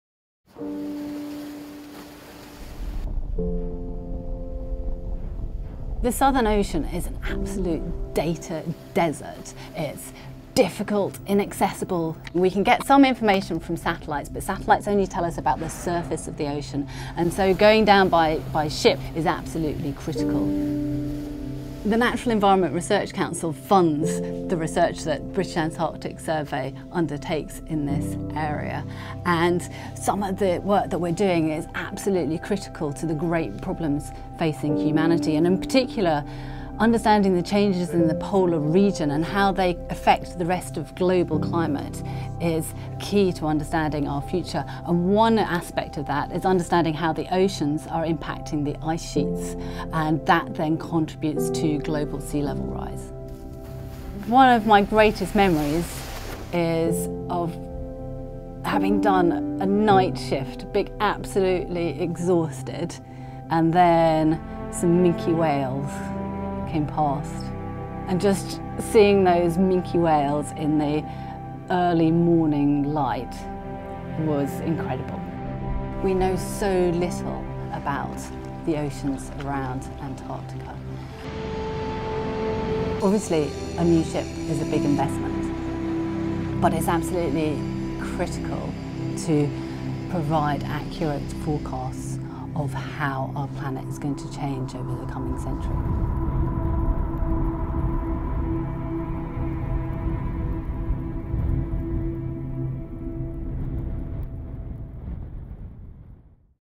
Name Our Ship Campaign: Emily Shuckburgh on the Importance of Antarctic Science
The polar region is one of the most inhospitable places on earth, but it is also one of the most important for our planet's future. In this video, Emily Shuckburgh from the British Antarctic Survey shares her experiences in Antarctica and the vital role of science in understanding the biggest problems facing humanity.

Filmed as part of our Name Our Ship campaign, this video explores the challenges of gathering data in the southern ocean, and how exploring by ship is critical to our understanding of the biggest problems facing humanity. From understanding changes in the polar region to forecasting how our planet will change over the coming century, the work being done in Antarctica is vital to our future.

Join us in this fascinating adventure to learn more about the importance of science in Antarctica.

About NERC